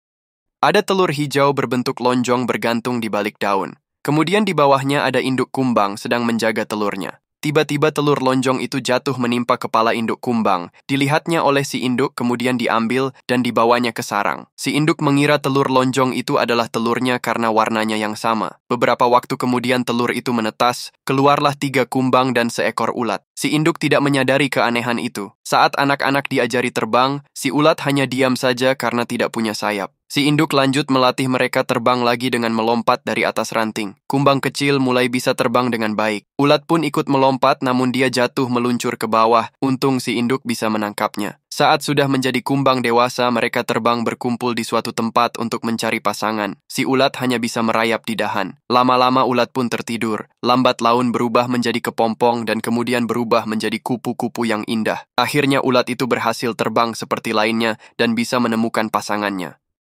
Terungkap: Kisah Haru Kumbang yang Salah Merawat Telur Kupu-kupu
"Selamat datang di Pinouno! Nikmati kisah penuh keajaiban dan inspirasi tentang sebuah telur hijau berbentuk lonjong yang mengubah hidup sekelompok kumbang dan ulat. Di tengah hutan yang subur, ada sebuah kejadian langka yang menggetarkan hati alam. Sebuah telur hijau lonjong yang menggantung dengan indah di balik sehelai daun, menjadi pusat perhatian induk kumbang yang berusaha melindunginya.

Tetapi, nasib tak terduga mengubah segalanya. Suatu hari, telur lonjong itu jatuh dan menimpa kepala si induk kumbang. Ia merasa terpanggil untuk merawatnya, percaya bahwa telur tersebut adalah miliknya karena warnanya yang serupa. Namun, ketika telur itu menetas, munculah tiga kumbang kecil dan seorang ulat.

Kisah ini menjadi semakin menarik saat si ulat, yang tidak memiliki sayap, mencoba belajar terbang bersama dengan saudara-saudaranya yang lebih beruntung. Mereka diajari untuk melompat dari atas ranting oleh sang induk, dan kumbang kecil mampu terbang dengan mahir. Namun, ulat terus berjuang karena ketiadaan sayap, hingga suatu hari terjatuh, namun beruntung diselamatkan oleh sang induk.

Setelah tumbuh menjadi kumbang dewasa, mereka semua berkumpul mencari pasangan hidup mereka, tetapi si ulat hanya bisa merayap di dahan. Namun, keajaiban alam pun datang saat si ulat tertidur, berubah menjadi kepompong, dan akhirnya menjadi kupu-kupu yang cantik.

Ini adalah kisah tentang perjuangan, transformasi, dan akhirnya, keberhasilan. Bagaimana si ulat yang dulunya tak berdaya akhirnya bisa terbang seperti saudara-saudaranya dan menemukan pasangan hidupnya adalah kisah yang menginspirasi kita semua untuk tidak pernah menyerah dalam menghadapi rintangan dalam hidup.

Tontonlah kisah ajaib ini dan nikmati keindahan alam serta pelajaran berharga yang bisa kita ambil darinya. Terima kasih telah menyaksikan!"

"Selamat datang di kisah luar biasa ini tentang sebuah telur hijau berbentuk lonjong yang menggantung di balik daun, dan petualangan penuh perjuangan dan transformasi yang akan menginspirasi hati Anda. Ketika telur tersebut jatuh dan menimpa kepala induk kumbang yang setia, dimulailah perjalanan yang tak terduga. Sang induk, dengan penuh cinta, mengambil dan membawa telur itu ke sarangnya, tanpa menyadari bahwa itu bukanlah telurnya sendiri.

Beberapa waktu kemudian, telur hijau itu menetas, menghasilkan tiga kumbang mungil dan satu ulat yang berbeda dari yang lainnya. Si ulat, tanpa sayap, adalah seorang yang istimewa dalam keluarga kumbang ini. Namun, dengan tekad yang kuat, mereka semua belajar terbang, dengan si ulat berusaha keras untuk ikut serta.

Di bawah bimbingan hangat sang induk, kumbang-kumbang kecil ini perlahan tapi pasti menguasai seni terbang. Tetapi si ulat, tanpa sayap, menghadapi kesulitan yang tak terbayangkan. Namun, keberuntungan berpihak pada mereka saat sang induk tanggap dan melindungi si ulat yang berani.

Ketika akhirnya mereka tumbuh menjadi kumbang dewasa, mereka berkumpul untuk mencari pasangan hidup. Namun, si ulat tetap dalam keadaan tanpa sayap, merayap di dahan sambil mengamati teman-temannya yang terbang bebas. Namun, sebuah keajaiban terjadi saat si ulat tertidur, berubah menjadi kepompong, dan akhirnya muncul sebagai kupu-kupu yang cantik.

Cerita ini adalah peringatan akan keajaiban transformasi dan kekuatan tekad dalam menghadapi perbedaan.